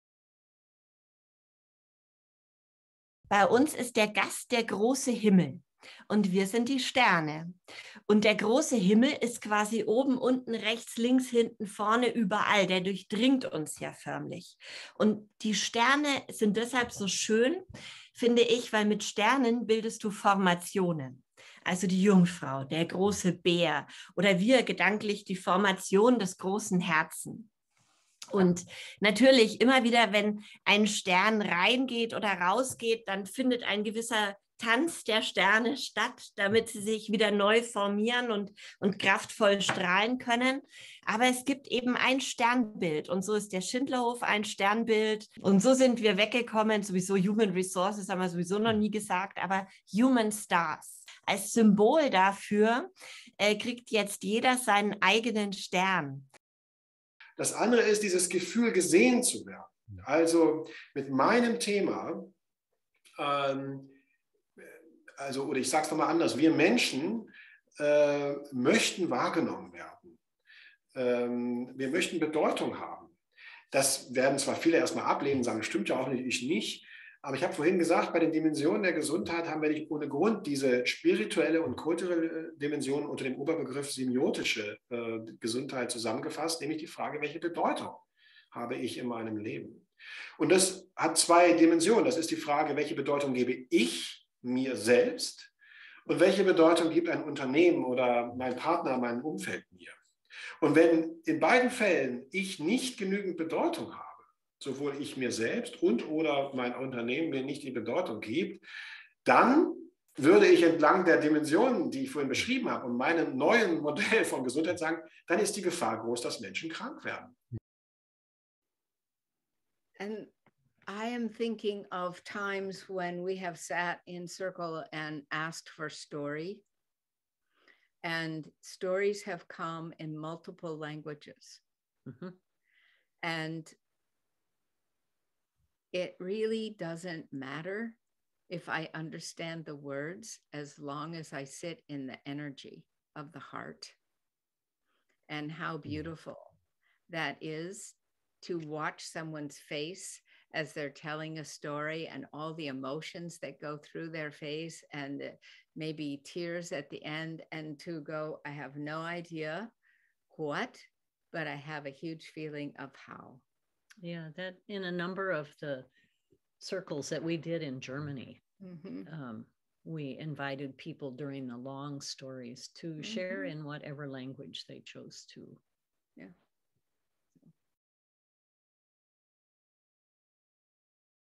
Tag 1 Sprecher:innen
Nicole Kobjoll, Prof. Dr. Tobias Esch und Ann Linnea mit Christina Baldwin. Die Sprecher:innen des ersten Kongress-Tages.